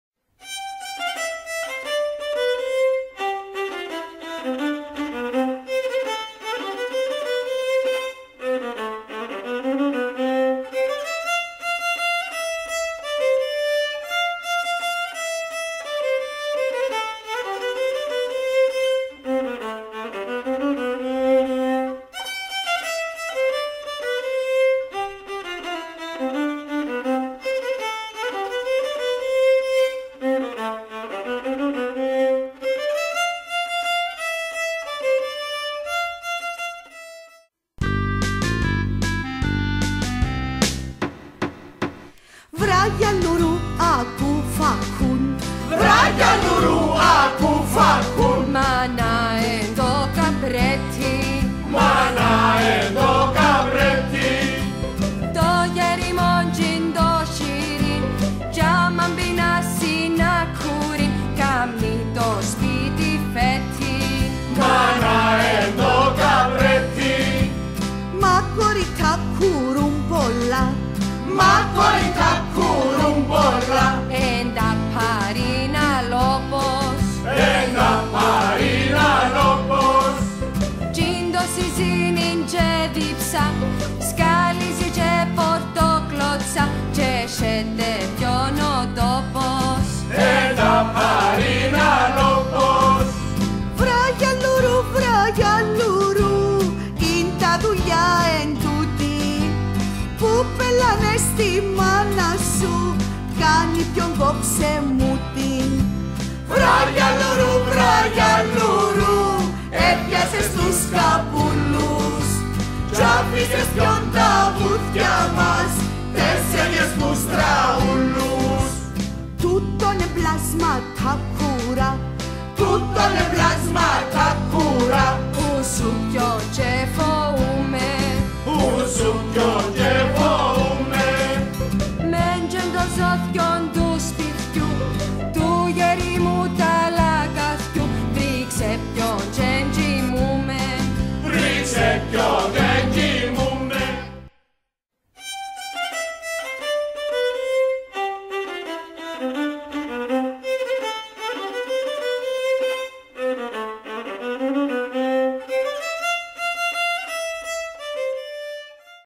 Θεατρική παράσταση "Η γιαλλουρού".wmv
O Θεατρικός όμιλος "Ιδάλια" Χαλκάνωρ Ιδαλίου παρουσιάζει την Θεατρική παράσταση "Η γιαλλουρού".
Κυριακή 6 Νοεμβρίου και ώρα 8 μμ. στο ΚΙΝΗΜΑΤΟΘΕΑΤΡΟ ΠΑΛΛΑΣ Λευκωσία Κύπρος
Διοργανωτής Κίνηση Γυναικών Ασώματου
Τα καθαρά έσοδα της παράστασης θα δοθούν στο Ίδρυμα Αρχιεπίσκοπος Λουκάς
Πολυδύναμος χώρος αγάπης για παιδιά με αναπηρίες.
Είσοδος €10